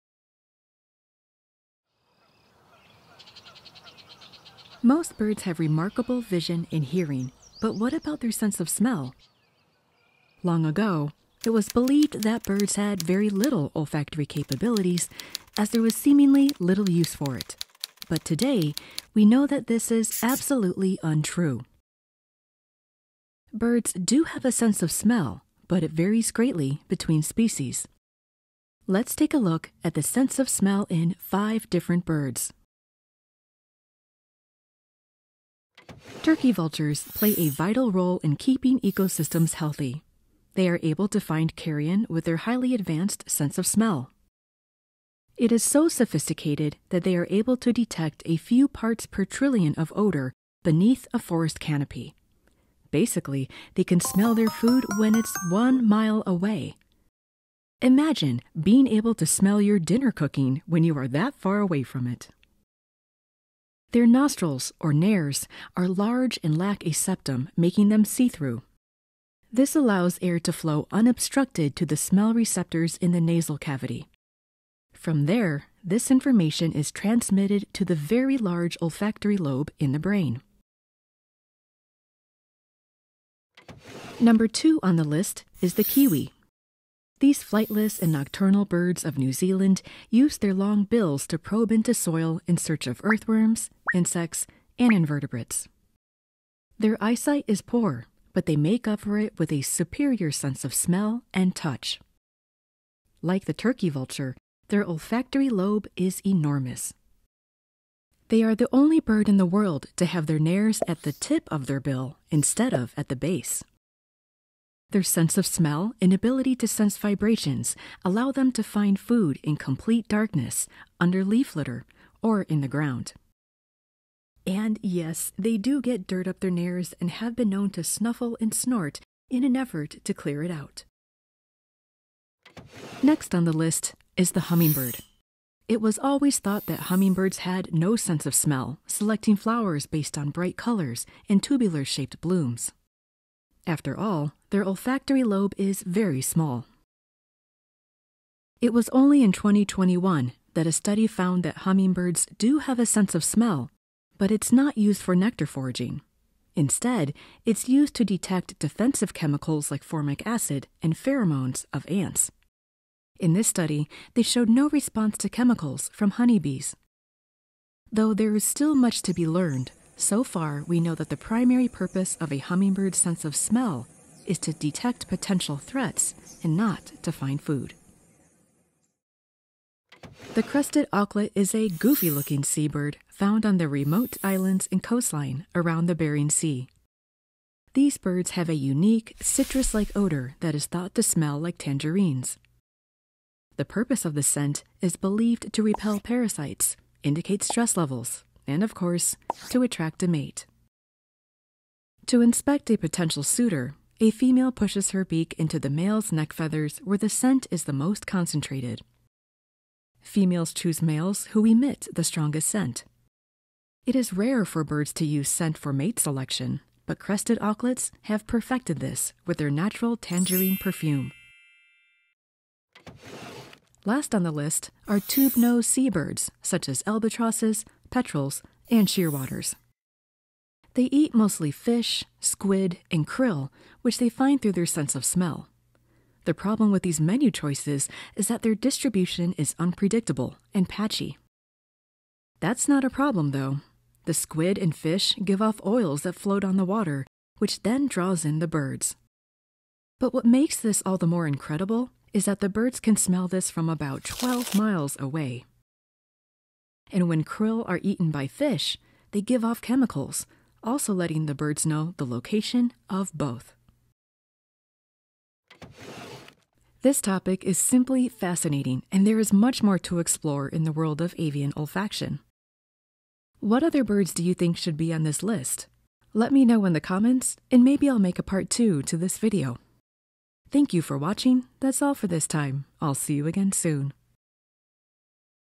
Do Birds Have A Sense Of Smell?
Most birds have remarkable vision and hearing, but what about their sense of smell? Long ago it was believed that birds had very little olfactory capabilities, as there was seemingly little use for it. But today, we know that this is absolutely untrue. Birds do have a sense of smell, but it varies greatly between species. Let's take a look at the sense of smell in five different birds.

Excellent book on birds and their sense of smell: The Secret Perfume of Birds

Chapters:
Do birds have a sense of smell? 00:00
Turkey Vulture 00:35
Kiwi 01:23
Hummingbirds 02:12
Crested Auklet 02:59
Tubenose Seabirds 03:42
Conclusion 04:25

Photos and video sources:
Canva.com

Videvo.net

Credits in order of appearance:
Introduction:
-Green heron by mlharing from Canva.com
-Sparrow in slow motion by andreas from Pixabay.com

Turkey vulture:
-Turkey vulture by Isaac Ruiz from Canva.com
-Turkey vulture soaring over forest by Wirestock from Canva.com
-Turkey vulture and see-through nares by Matt Cuda from Canva.com
-Turkey vulture video by Lukassek from Canva.com

Kiwi:
-Southern brown kiwi by Glen Fergus from Flickr.com
-Kiwi and egg by LuPa Creative from Canva.com
-Okarito brown kiwi by Mark Anderson from Wikipedia.com
-Kiwi in person's arms by Liu Yang from Canva.com
-Kiwi in leaf litter by powerofforever from Canva.com
-Kiwi crossing road sign and rainforest by tobiasjo from Canva.com
-North Island brown kiwi by The.Rohit from Flickr.com

Hummingbird:
-Anna's hummingbird by Dean Diemert from Pexels.com
-Anna's hummingbird by NNehring from Canva.com
-Hummingbird and bees by Juancarlos Córdova from Pexels.com
-Hummingbird and ants by pascalmarchand from Canva.com

Crested Auklet:
-Crested auklet by Gregory "Slobirdr" Smith from Flickr.com
-Crested auklet, background removed, by US Fish and Wildlife Service from Flickr.com
-Crested auklet colony by Alaska Region Fish and Wildlife Service from Flickr.com
-Crested auklet pair by US Fish and Wildlife Service from Flickr.com
-Crested auklets on rock cliff by NOAA Photo Library from Flickr.com
-Crested auklet flock in flight by AGAMI Stock from Canva.com

Tubenose Seabirds:
-Wandering albatross parent and chick by BlackBoxGuild from videvo.net
-Sooty shearwater by Manakin from Canva.com
-Wilson's storm petrel by birdsonline from Canva.com
-Black-browed albatross and giant petrels by SmallCoot from Canva.com
-Giant Petrel by mauinow1 from Canva.com
-Black-browed albatross by SmallCoot from Canva.com

Conclusion:
-Copper-rumped hummingbird on heliconia flower by Rick Ray from videvo.net

Thumbnail credit: Garden emerald hummingbird by Steven Paton from Pexels.com